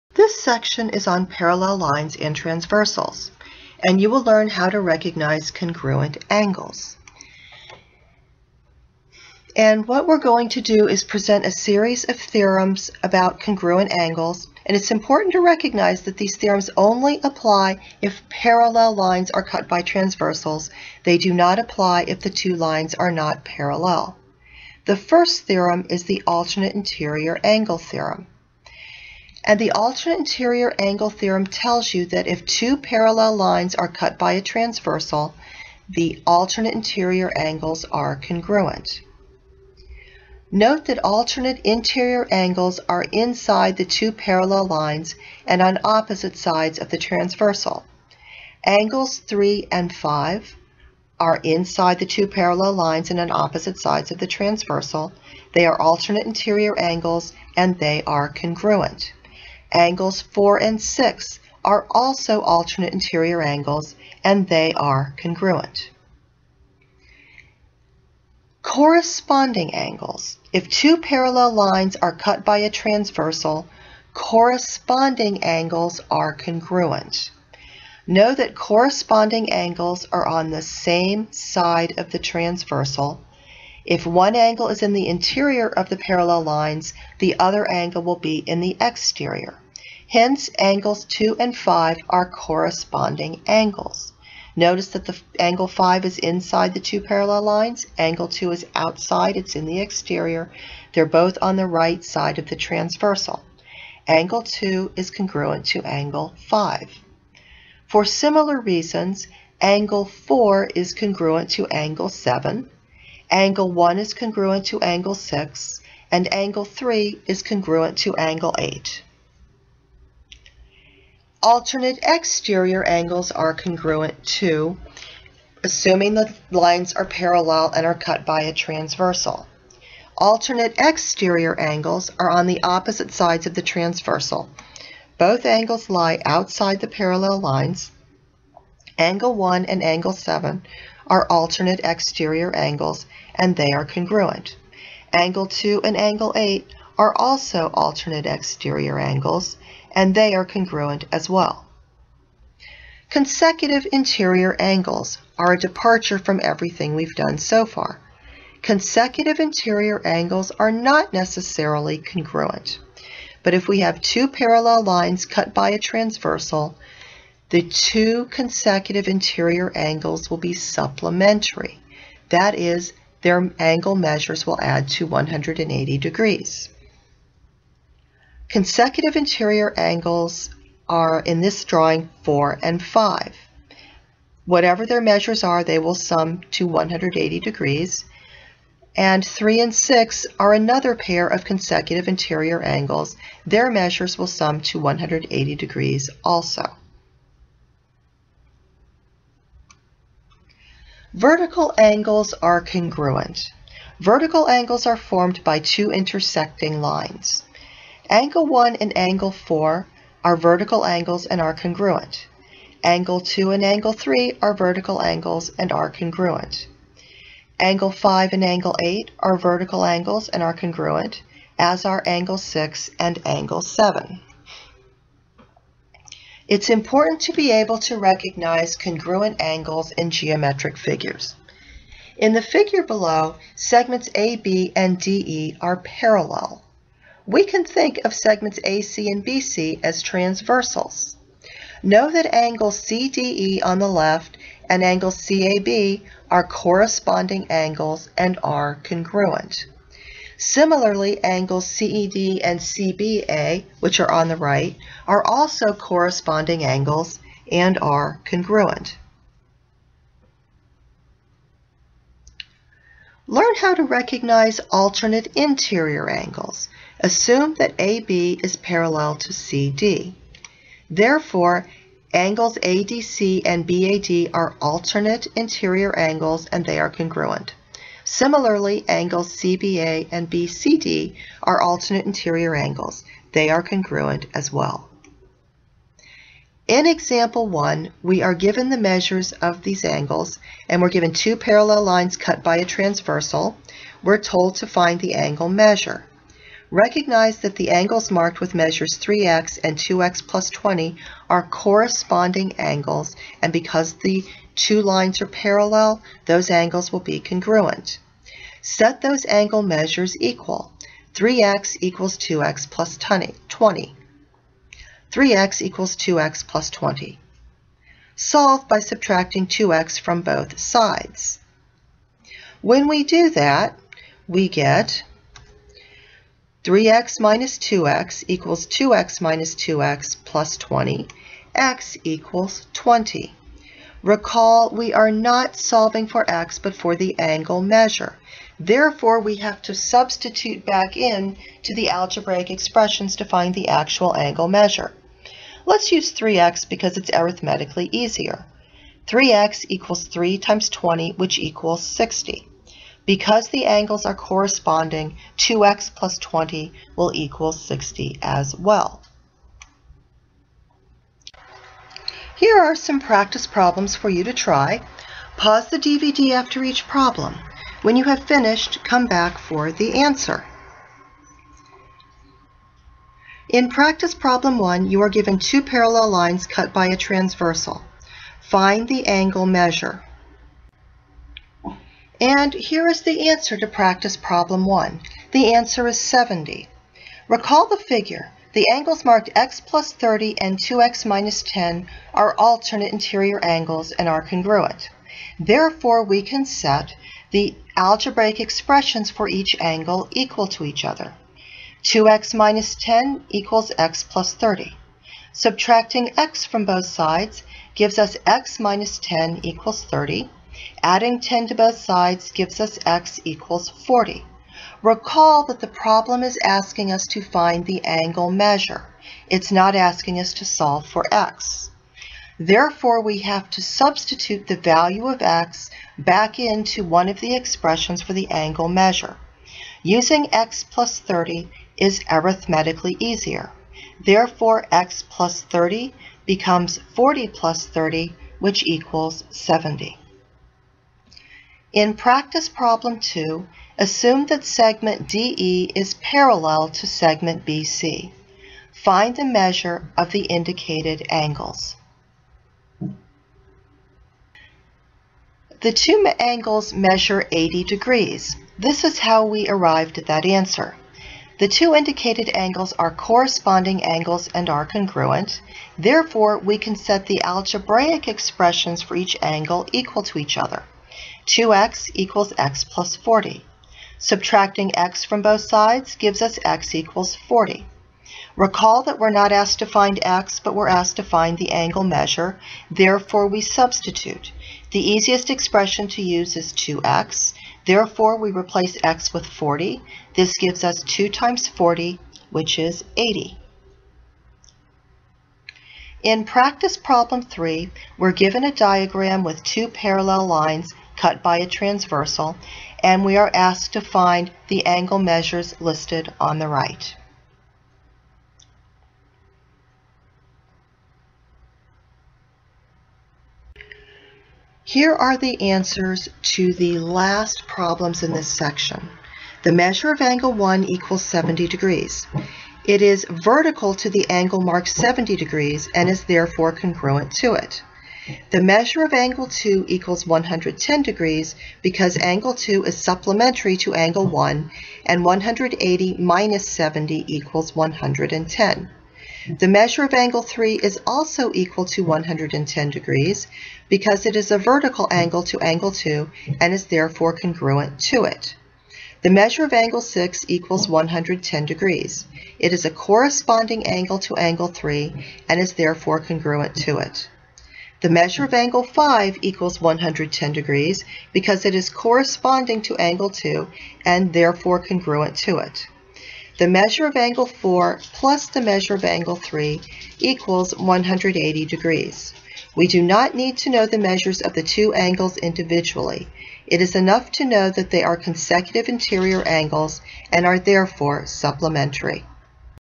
Free SAT Math Lessons: Parallel Lines and Transversals
Nearly all SAT Math tests have at least one problem on parallel lines and transversals.  Fortunately, the problems are usually simple--and a great way to earn easy points.